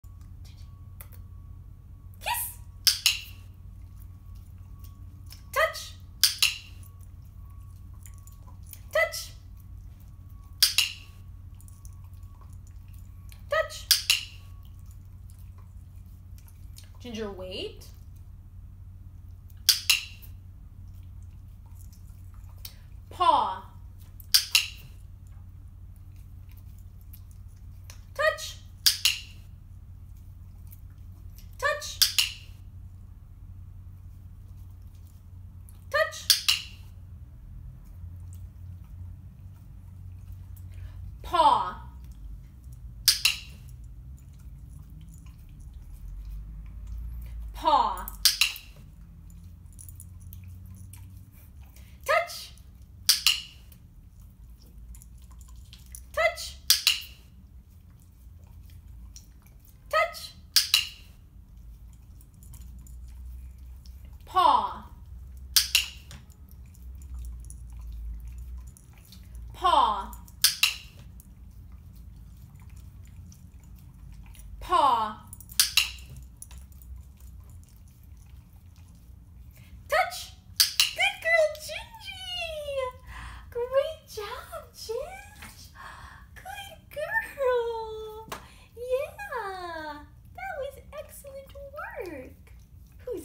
Verbal discrimination
Anna demonstrates verbal discrimination. Same object, different behaviors, based on the verbal cue given.